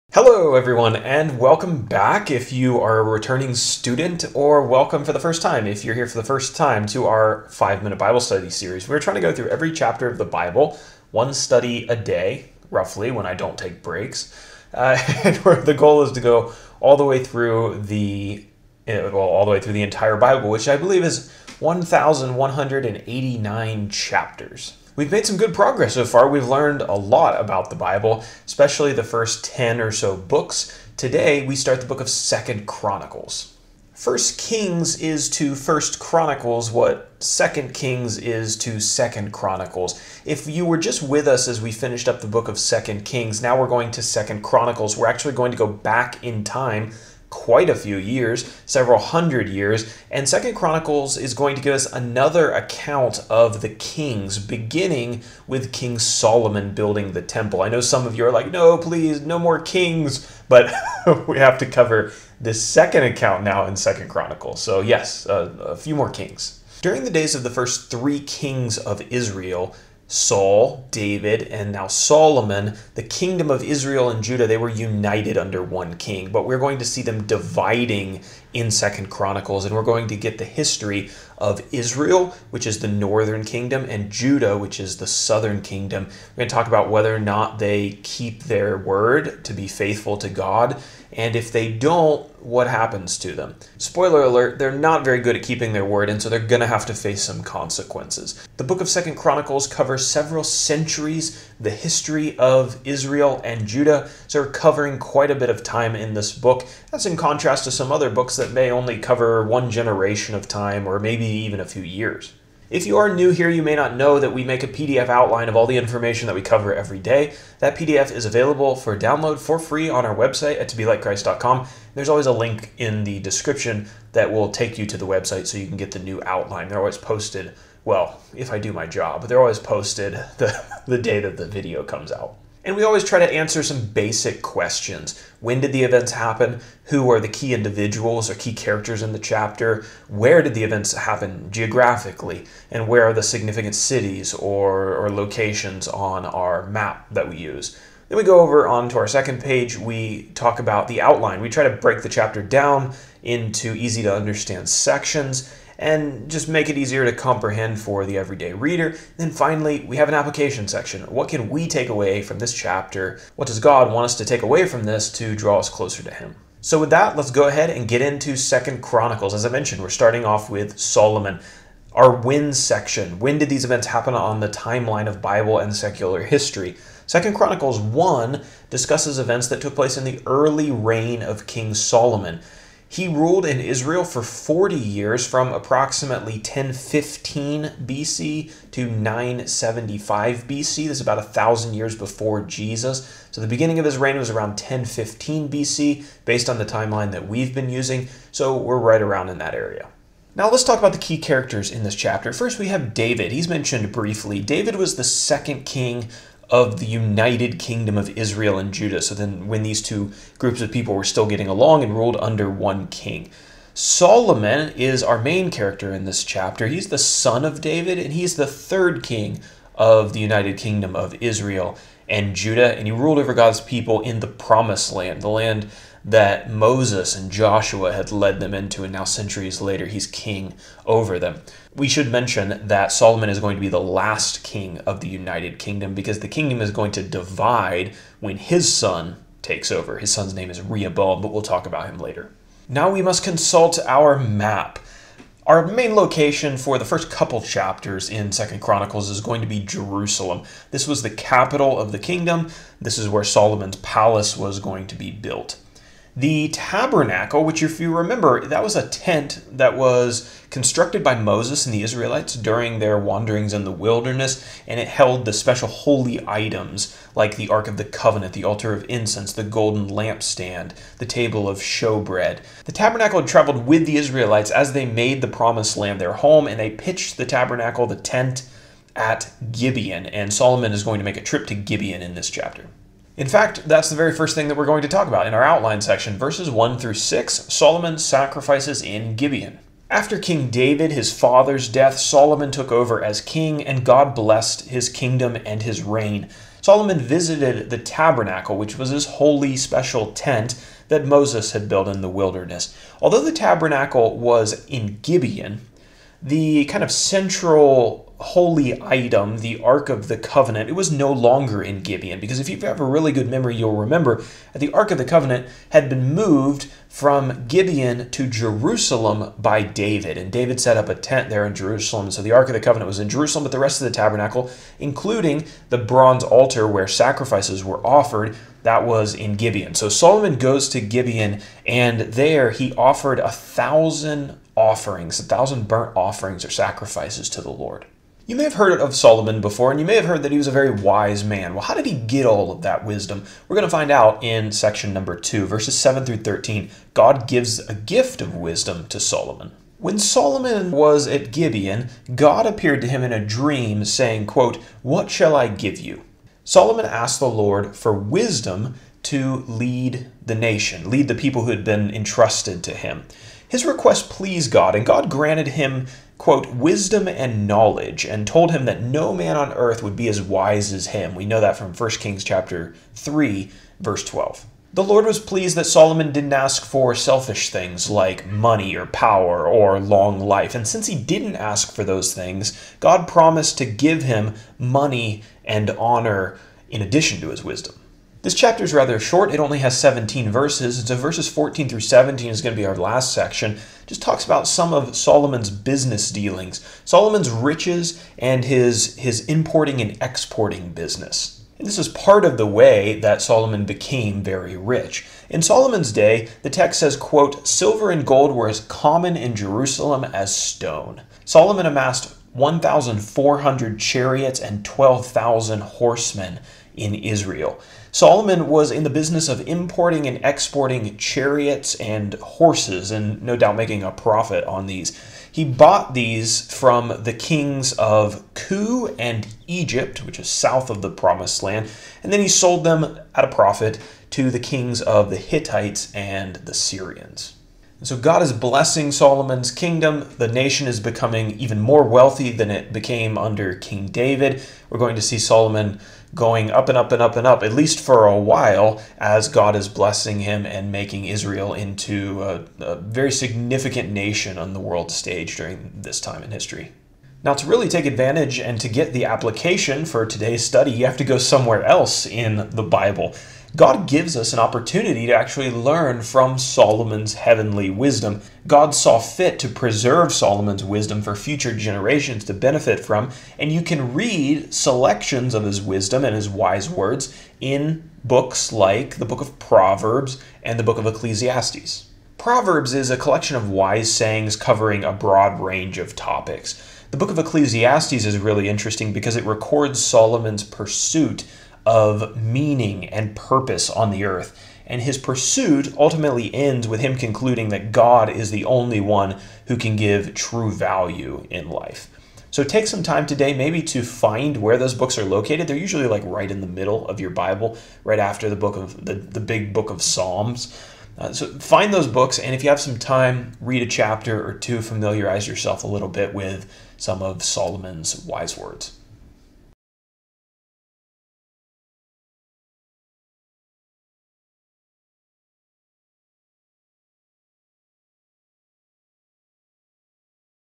2 Chronicles 1 Summary: 5 Minute Bible Study
Get a quick overview of 2 Chronicles 1, including its themes and key events. This article provides a short summary of 2 Chronicles chapter one, and will help you understand the book of 2 Chronicles as a whole.

In 2 Chronicles 1, Solomon becomes king of Israel after the death of his father, David. While Solomon was offering sacrifices at the Tabernacle in Gibeon, God appeared to him and told him He would grant him a request. Solomon asked the Lord for wisdom to lead the nation. God granted Solomon wisdom superior to any man on earth.